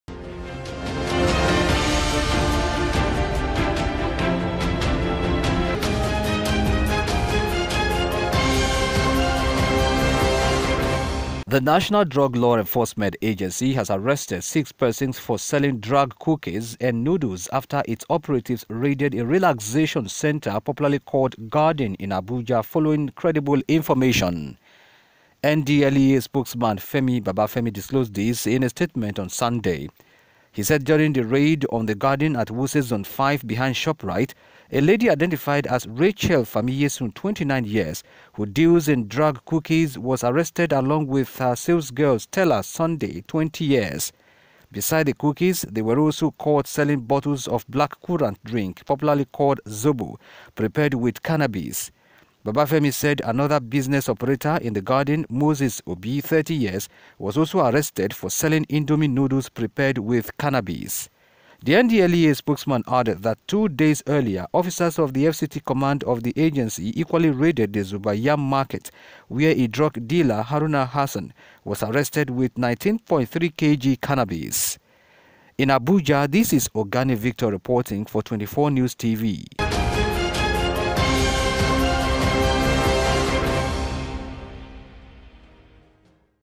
DRUG ABUSE
NDLEA Raids FCT, Arrests 6, Trails Others